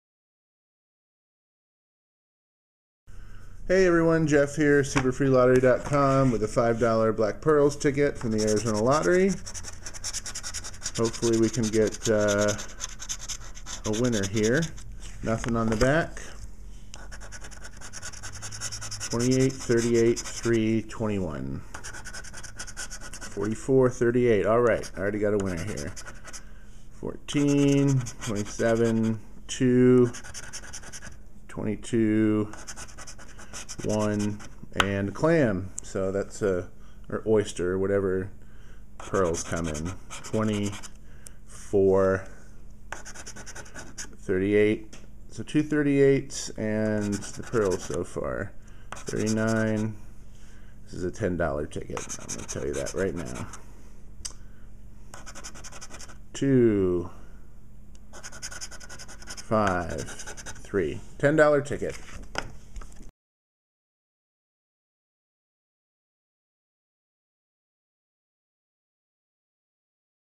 $5 Black Pearls Scratcher
This is a cool $5 Black Pearls scratch off ticket from the Arizona Lottery